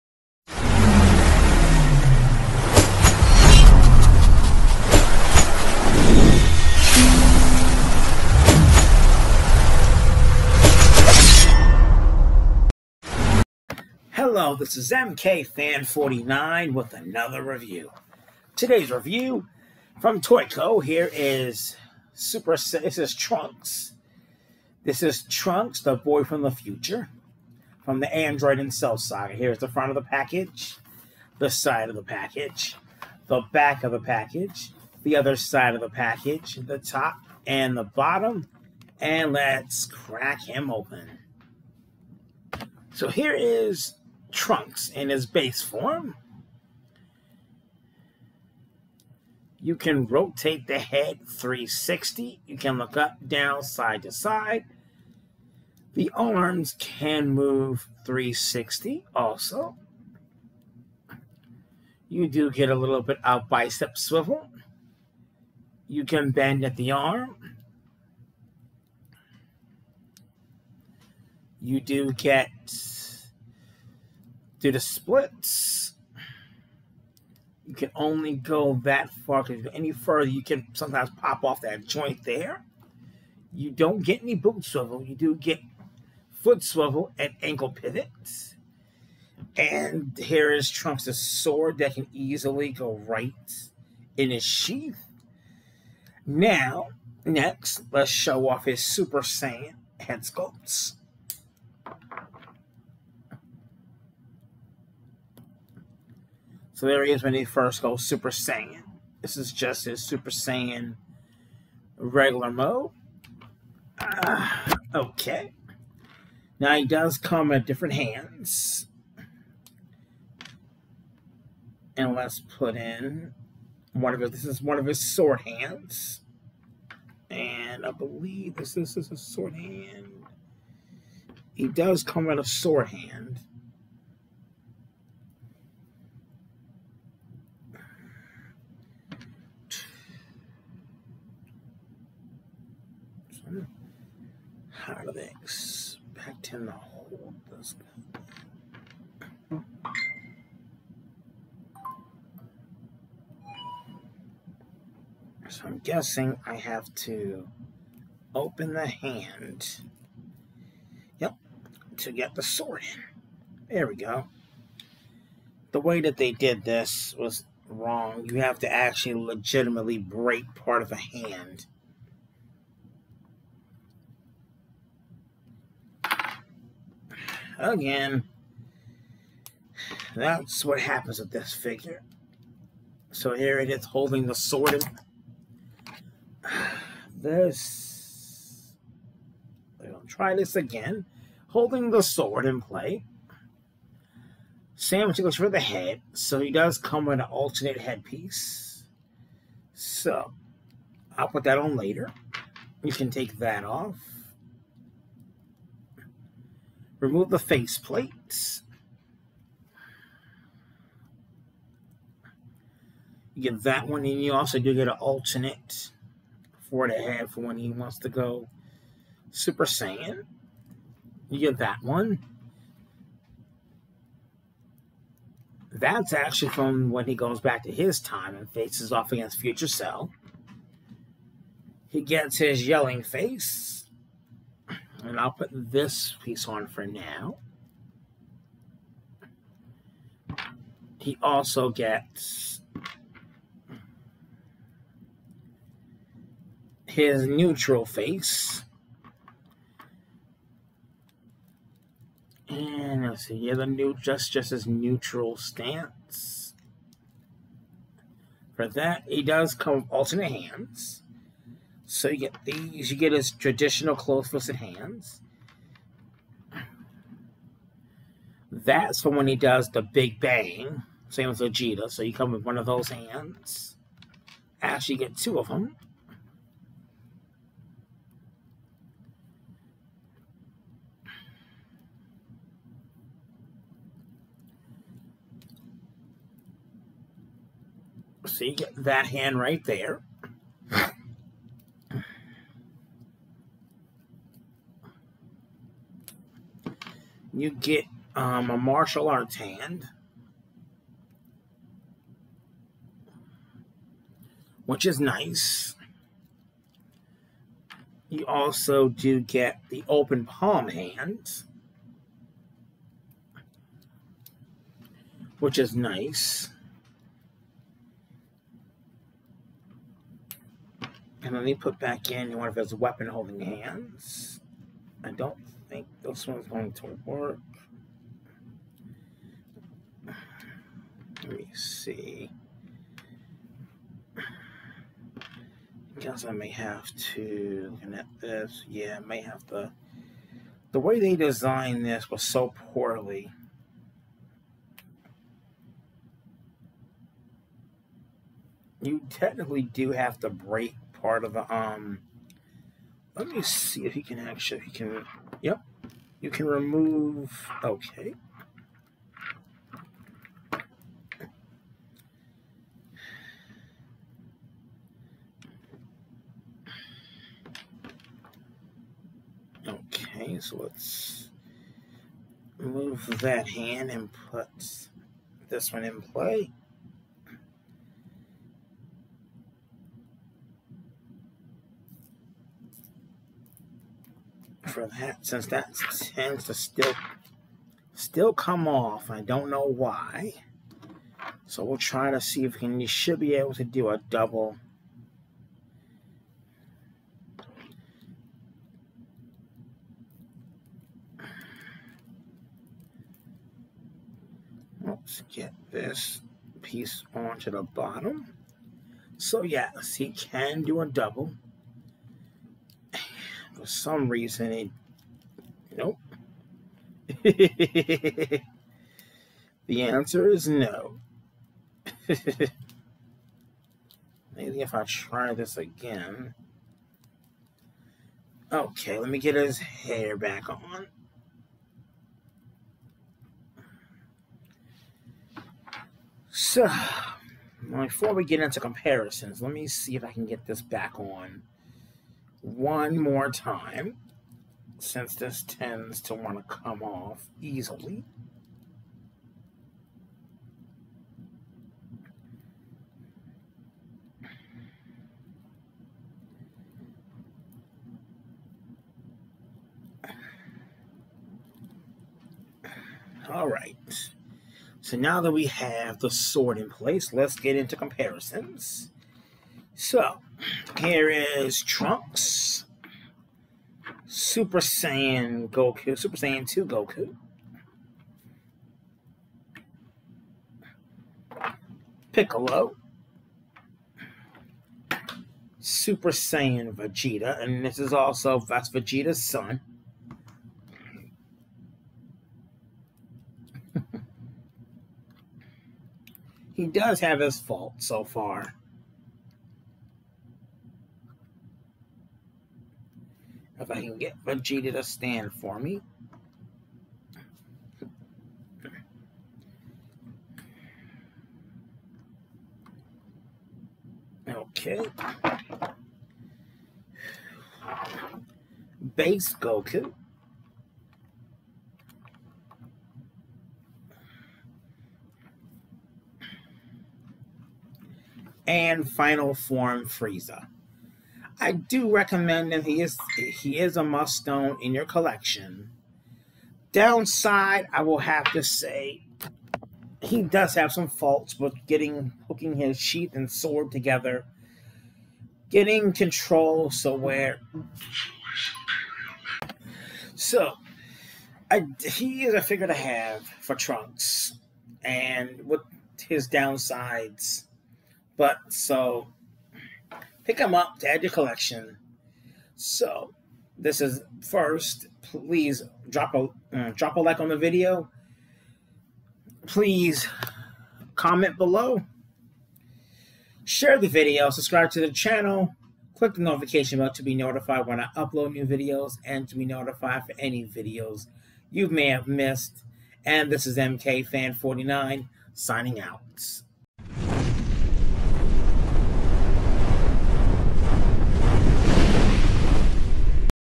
SHFiguarts Dragon Ball Z Trunks The boy from the future
This is a review on SHFiguarts Dragon Ball  Z Trunks The boy from the future